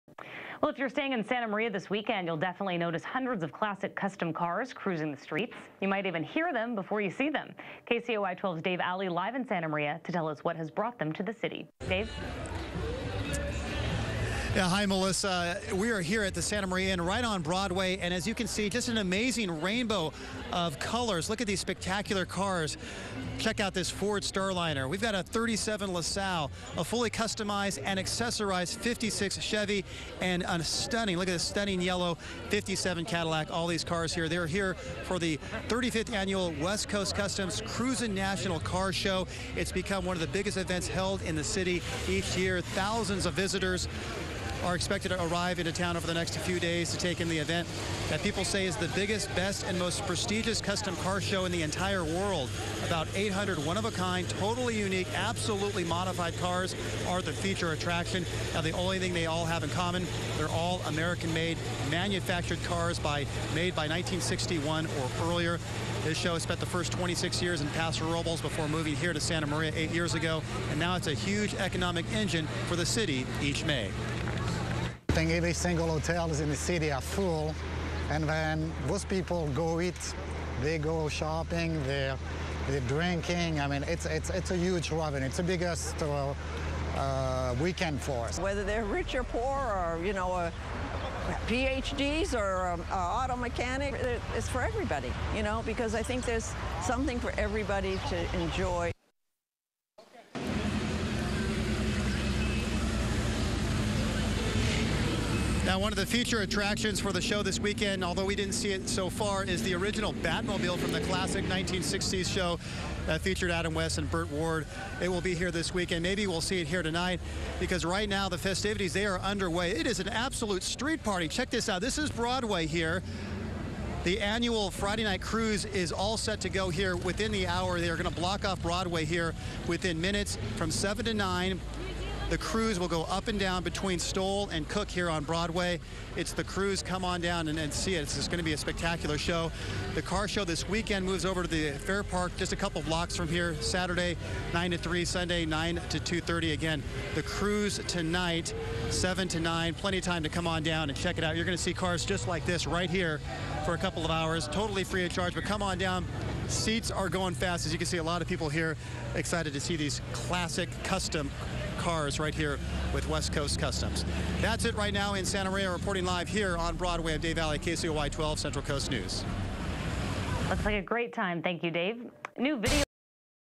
Cruisin' Nationals Rolls Into Santa Maria
The popular West Coast Kustoms Cruisin' Nationals car show annually draws thousands of visitors to Santa Maria providing a huge economic boost to business in the city.